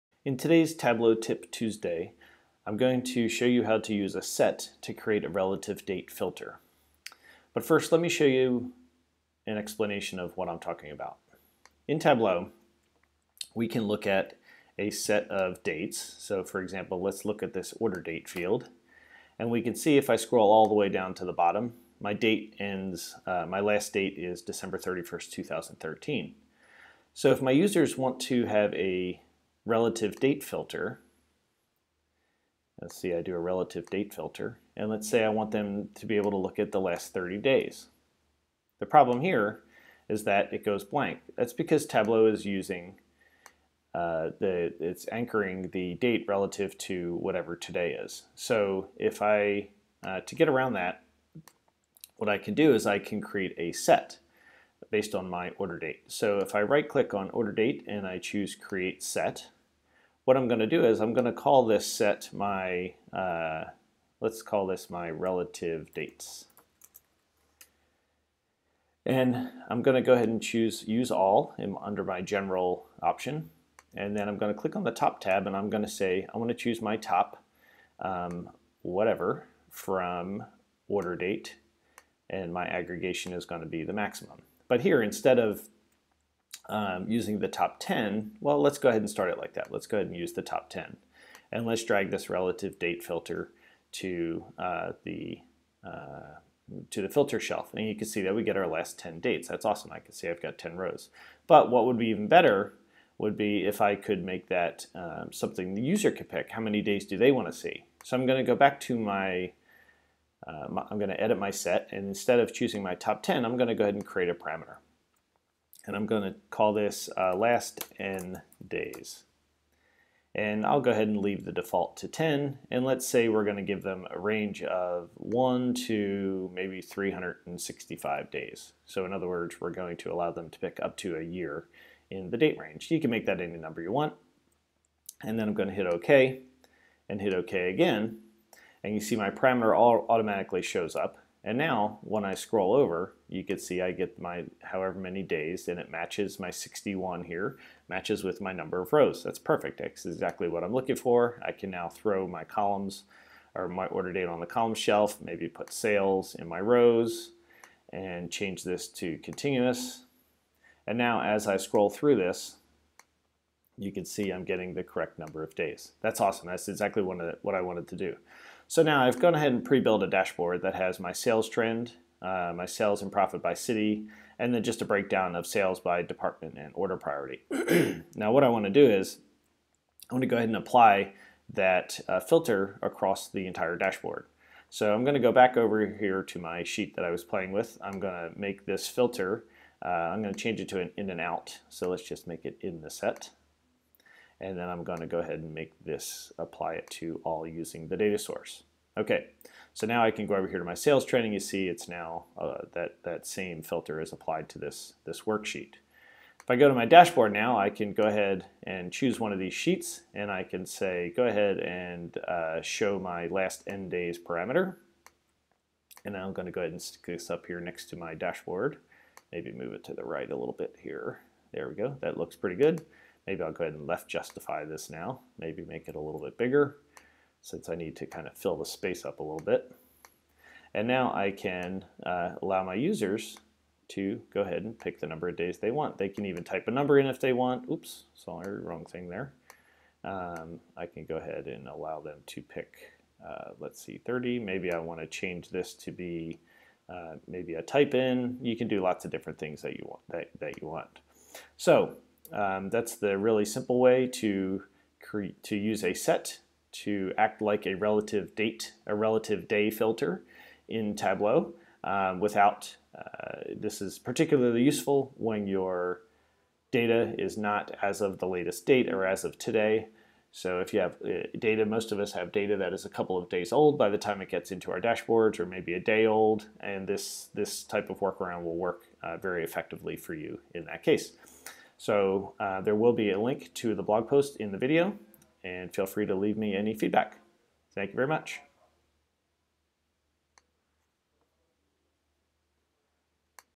Using a Set to Create a Relative Date Filter
This week's Tableau Tip Tuesday shows you how to use a set to act like a relative date filter. This is particularly effective when your data is not always current.